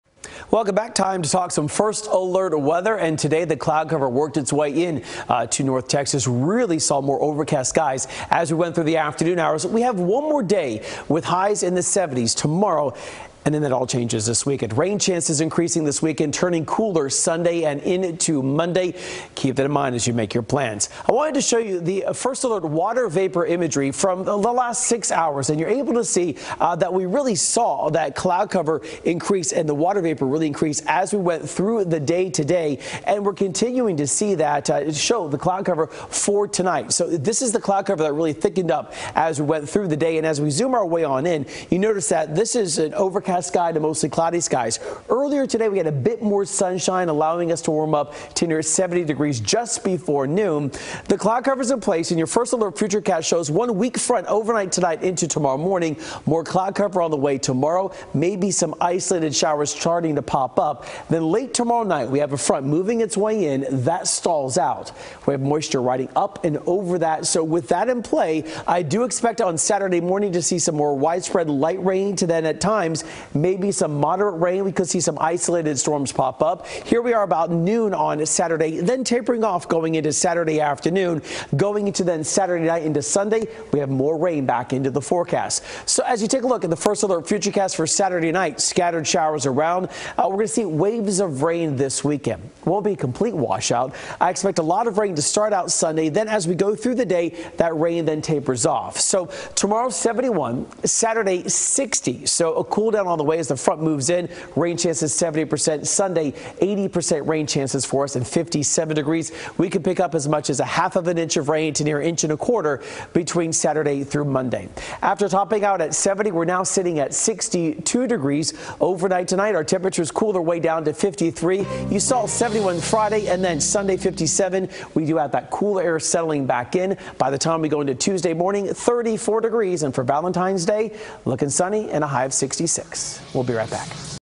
One more day with highs in the 70's ahead of cooler temps
Rain chances increase this weekend and we get cooler weather on Sunday and Monday.